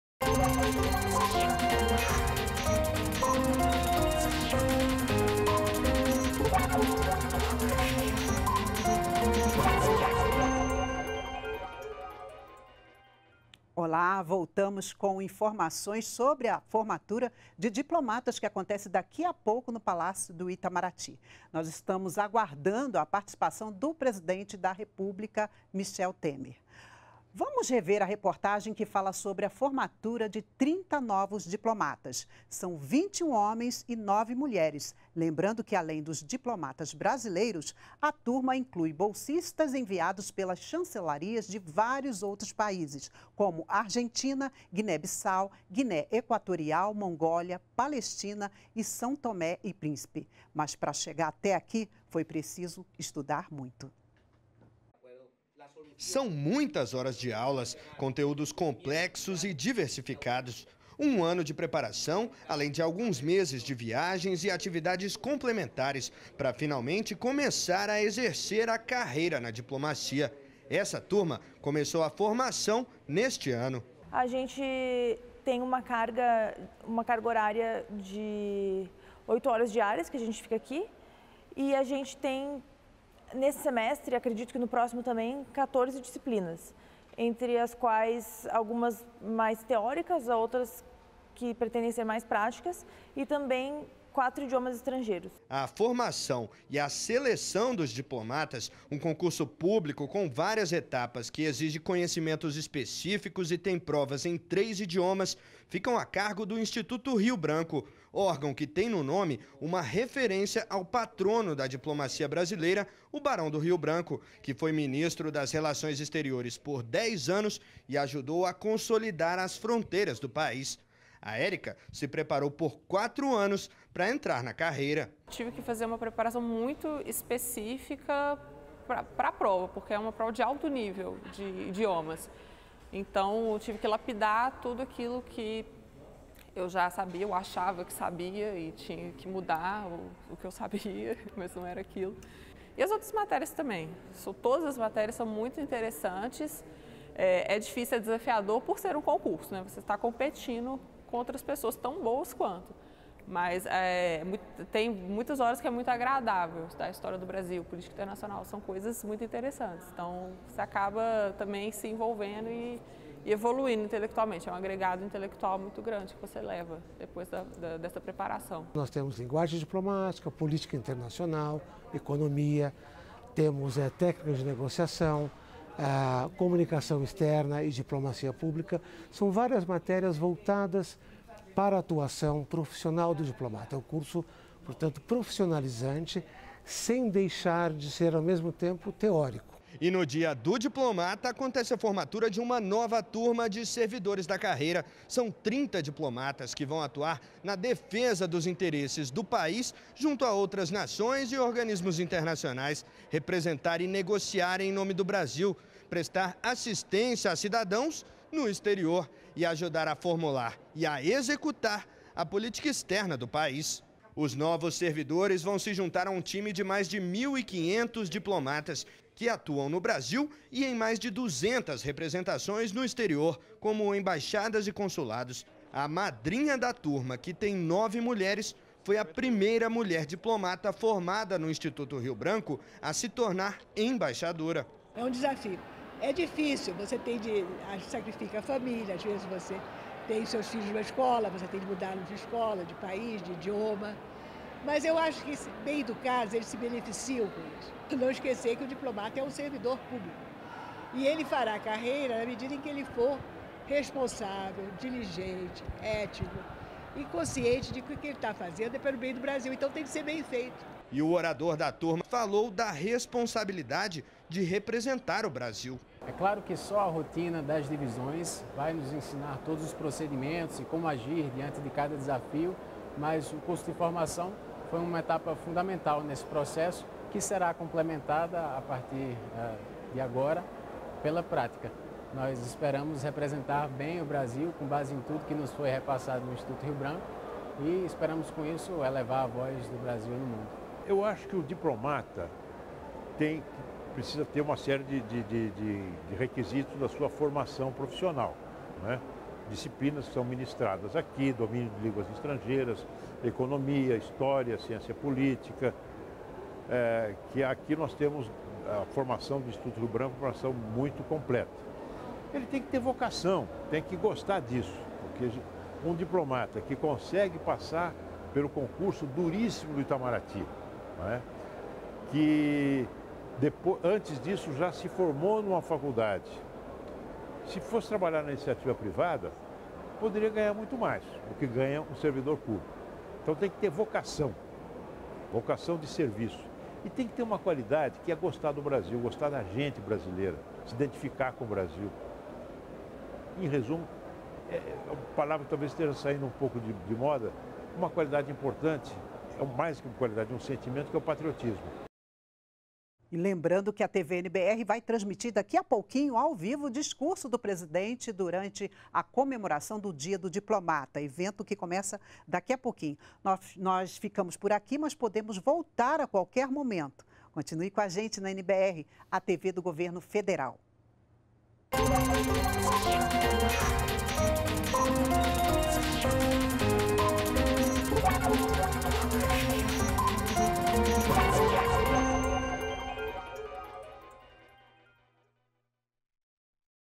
Presidente Temer vai participar da cerimônia de formatura de 30 novos diplomatas
REPÓRTER NBR - 20.04.18: nesta sexta-feira (20), acontece no Palácio do Itamaraty, a formatura de 30 novos diplomatas. O presidente Michel Temer vai participar da cerimônia.